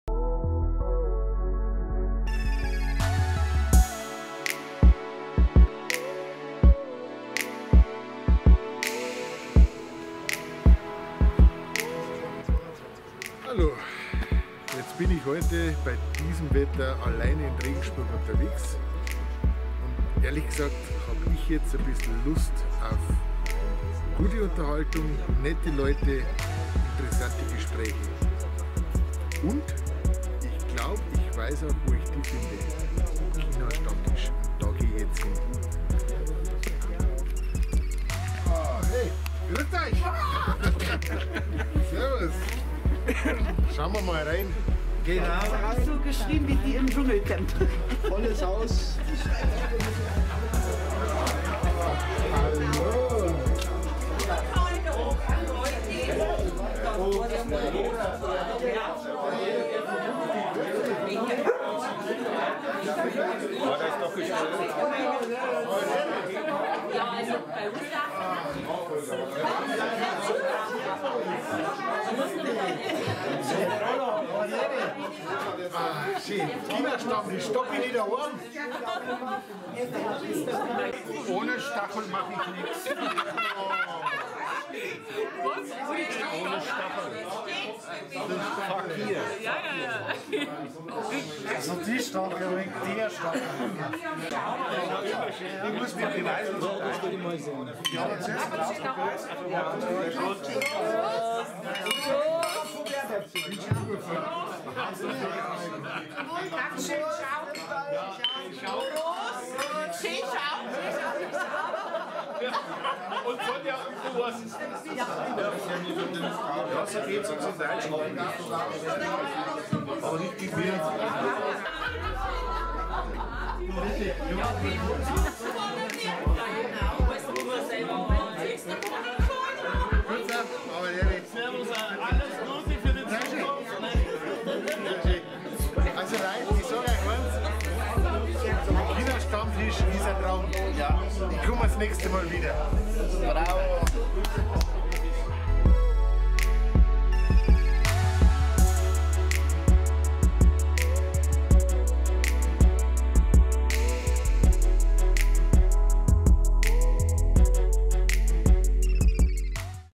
MandlFilm präsentiert - Der regensburger Chinastammtisch
Der regensburger Stammtisch, ideal für gute Gespräche und nette Leute
Gefilmt und bearbeitet von MandlFilm
wer ebenfalls ein Werbevideo von sich, seiner Veranstalten möchte, einfach bei mir melden
Mandl Holger bei Facebook